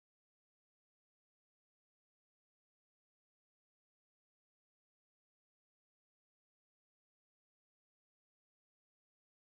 1578 Business people are discussing in the conference room.
We see emotions on their faces during brainstorming.

Adventurers, Filmmakers, Timelapses, Camera Car, Aerials High Quality Stock Footage, Warsaw based.